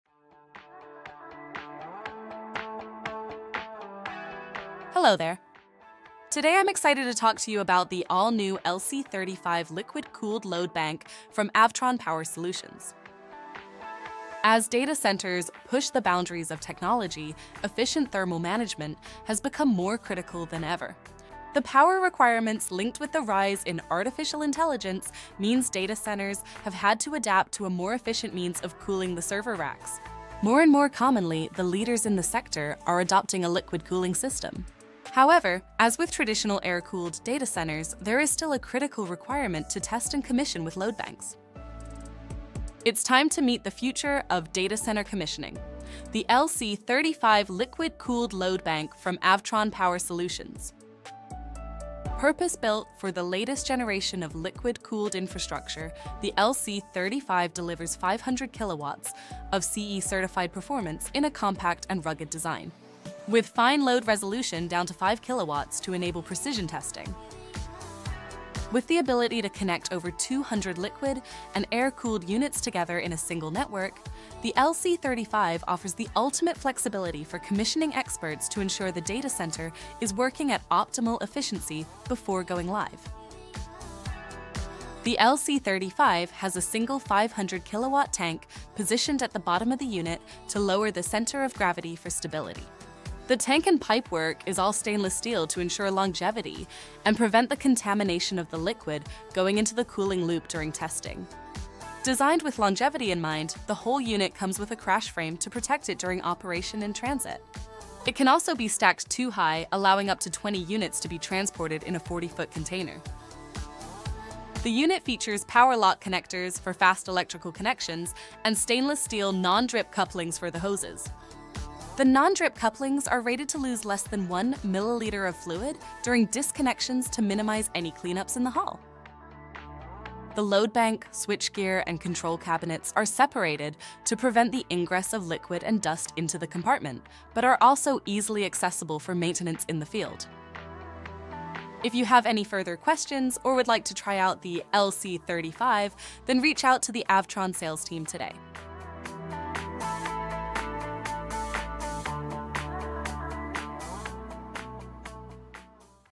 LC35 Liquid Cooled Load Banks | Features & Benefits
Discover the power, precision, and performance of the LC35 Liquid Cooled Load Bank from Avtron Power Solutions. In this video, we explore the key features, technical advantages, and application benefits that make the LC35 the ideal choice for commissioning liquid-cooled data centers.

Key highlights include:

✅ Compact and scalable design
✅ 500kW in a small footprint
✅ Seamless integration with liquid cooling infrastructure
✅ Rugged construction for mission-critical environments
✅ Advanced control options for precision testing

🔧 Engineered for performance. Built to last.